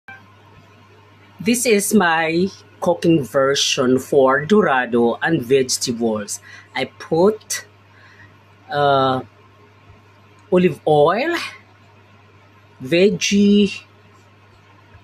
Dorado and Vegetables in Oven!!Healthy Menu!! Short!!!!
Olive Oil,Paprika,potato,zuchinni,black pepper,salt and carrots,,,,